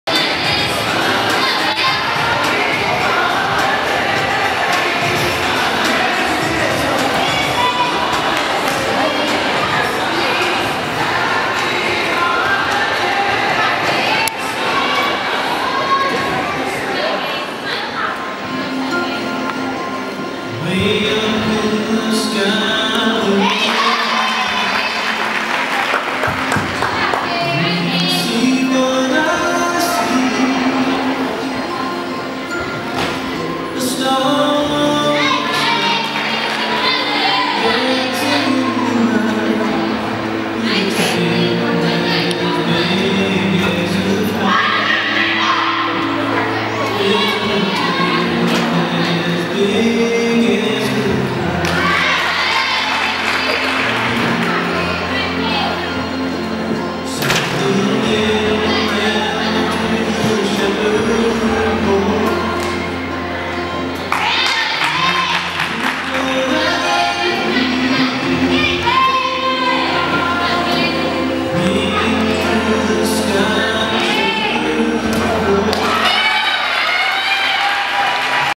Paisley Reed - Beam - 12/4/10
Paisley Reed...
Beam...
Red Team...

Fans of TWU gymnastics got their first look at the 2011 Pioneer team at the annual Intrasquad Holiday Spectacular on Saturday night at Kitty Magee Arena.Nearly the entire squad took part in the meet, which was split into to two teams, Red and Green. The two sides competed on vault, uneven bars, balance beam and floor exercise.